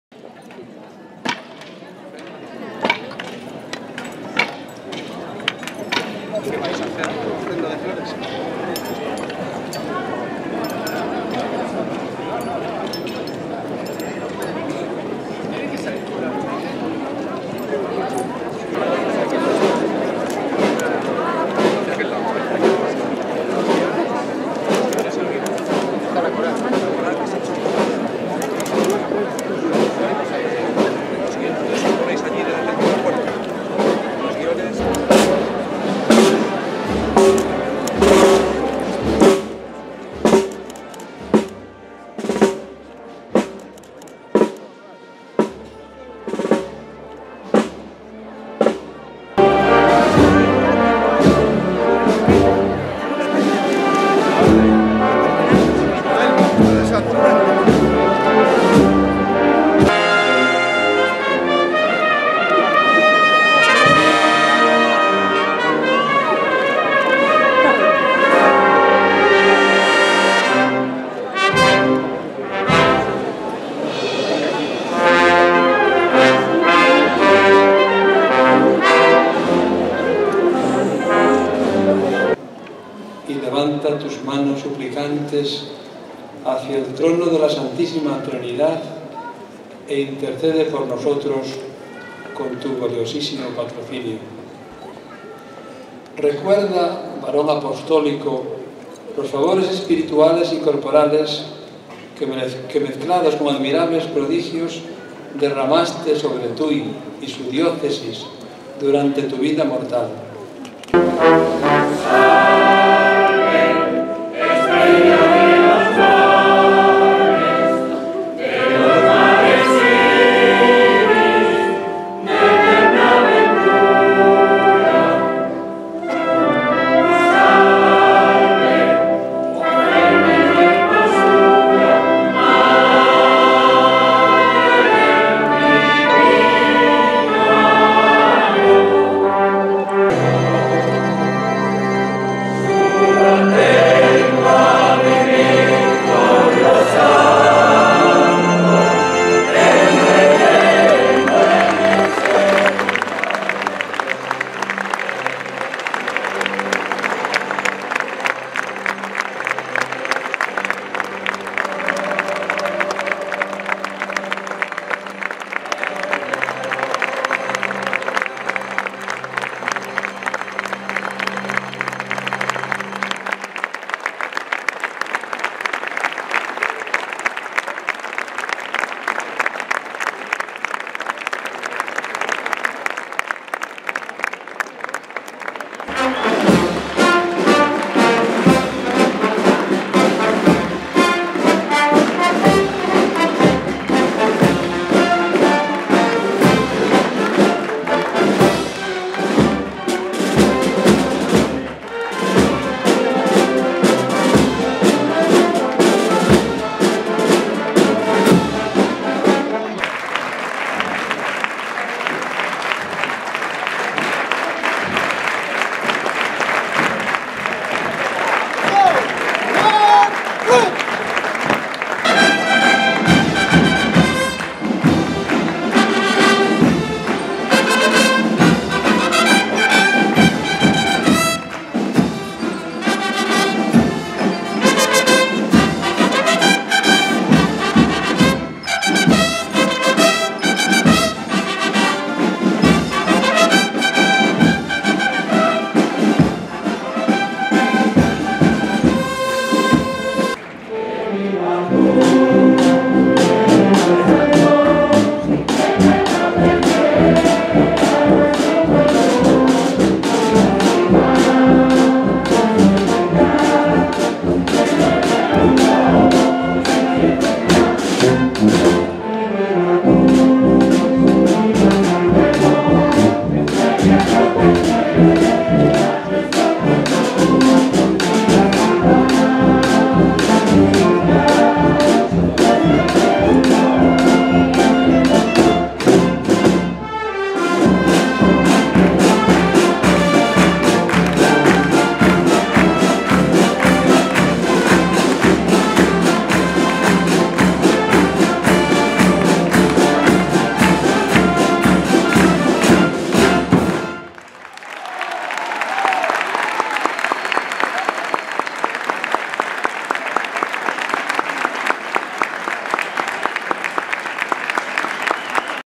Tuy. Procesión de San Telmo 2014
Procesión de San Telmo en Tuy, Galicia, el 28 de abril de 2014. Contó con el acompañamiento musical de la Banda de Música de la Escuela Naval Militar de Marín, la Banda de Guerra de la BRILAT y la Banda de Música Popular de Tuy. También participaron los Bombeiros Voluntários de la vecina localidad portuguesa de Valença do Minho y el presidente de la Xunta de Galicia, Alberto Núñez Feijóo. El paso de San Telmo estuvo escoltado por miembros de la tripulación del Patrullero "Cabo Fradera" (P-201), el segundo barco más antiguo de la Armada Española, que tiene su base en la Comandancia Naval del Miño, en Tuy.